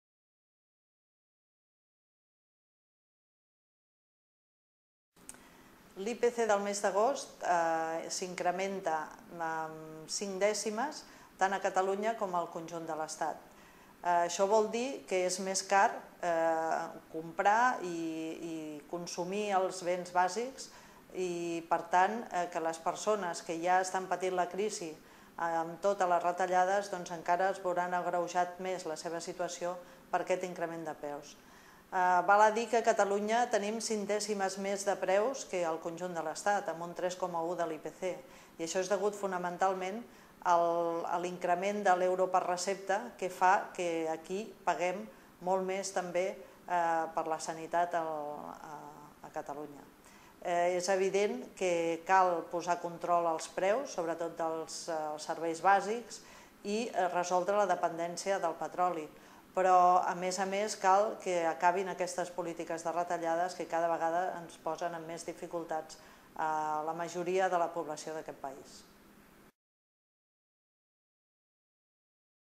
Valoració de les dades de l'IPC de l'agost de 2012
Dolors Llobet, secretària de Comunicació i portaveu de CCOO de Catalunya, valora les dades de l'Índex de Preus al Consum del mes d'agost de 2012.